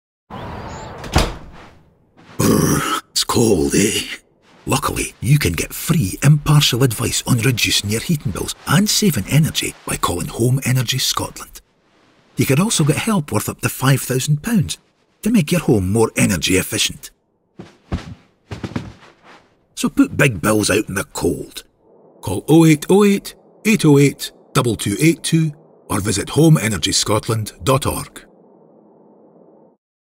Reduce bills and save energy with Doug the Caterpillar from Home Energy Scotland
Get free and impartial advice on how you can reduce your heating bills and save energy by contacting Home Energy Scotland today. You could also get support and funding like an interest-free loan with cashback or help worth up to £5,000 to make your home warmer and more energy efficient.